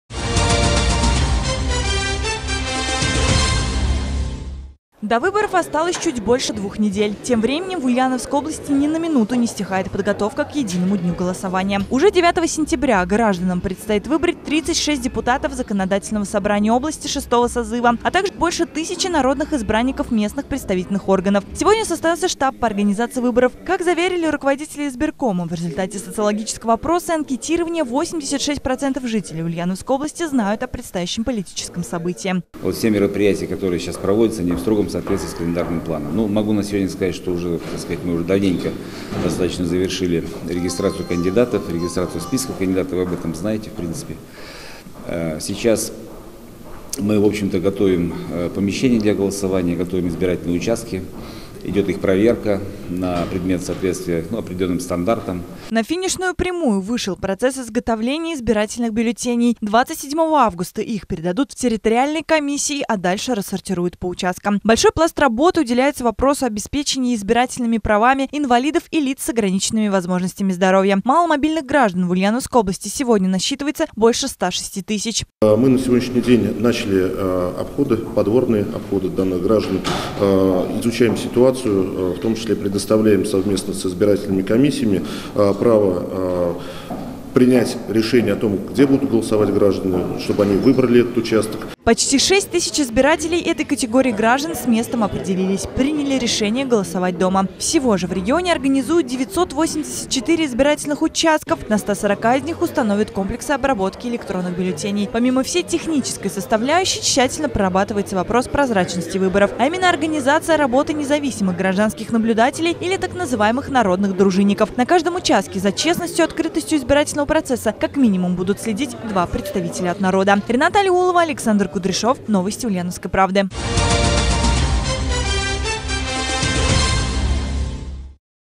Кандидаты в ожидании, бюллетени готовы. В Ульяновской области продолжается подготовка к выборам
До выборов осталось чуть больше двух недель. Тем временем в Ульяновской области ни на минуту не стихает подготовка к единому дню голосования.